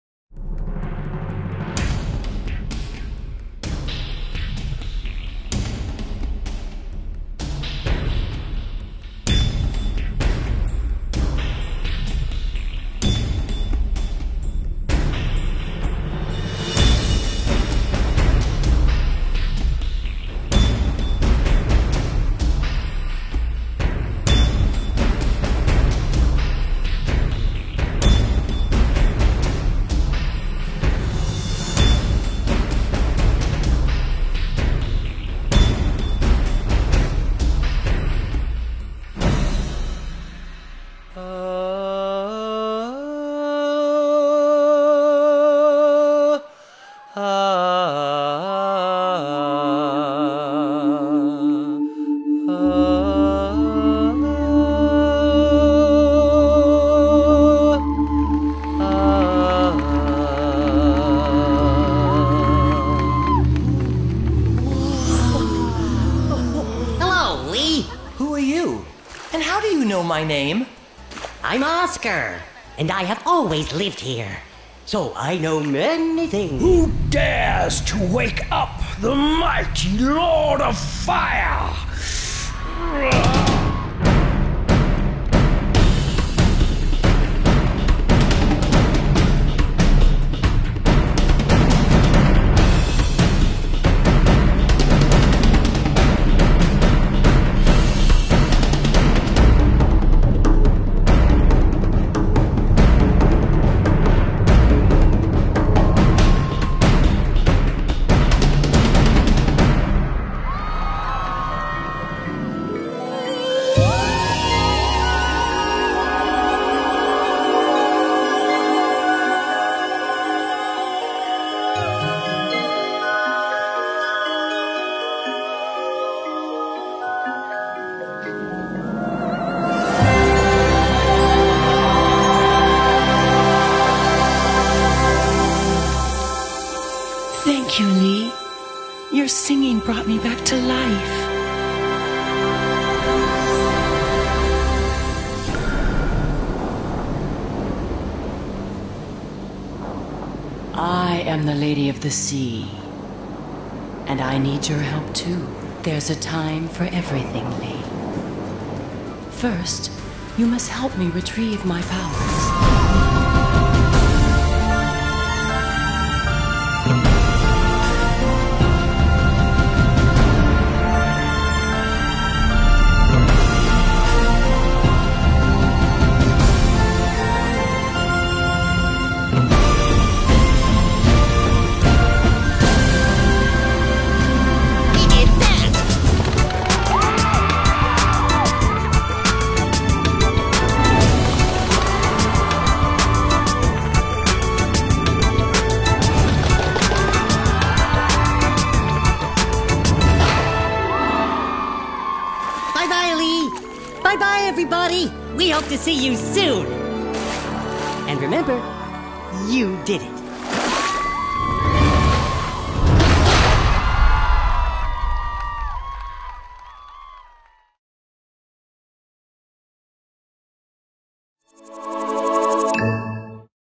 SONGS OF THE SEA SENTOSA ISLAND SINGAPORE
The unique scenography of this spectacular consists of a 120 meters long traditional fisherman village sets in the sea that plays host to an array of multimedia effects. Three waterscreens, including a 50 meters wide one; 69 water jets of 40, 20 and 16 meters; 19 flames, pyrotechnic, 7 lasers, 480 light fixtures, video, sound and mist are combined with 7 live performers to tell a story of magic and music.

A young man gifted with a beautiful voice, goes on a quest to free a princess from an evil spell. Helped by his friends, strange sea creatures, and impressive spirits, he will sing his way through every obstacle to achieve his goal for the pleasure of 6000 spectators every evening.

CREDITS:
Design and Direction: Yves Pépin
Scenario and artistic coordination: Moïra Smith
Music and Soundtrack: Philippe Villar / Pascal Lengagne
Cartoons and Animation: Dominique Fages / Gilbert Louët
Laser Design: Claude Lifante
Content Production: Thierry Nutchey
Project Manager: Carolina Jara Pinzon
Technical Director: Guillaume Duflot
Production: Jean Christophe Canizares / ECA2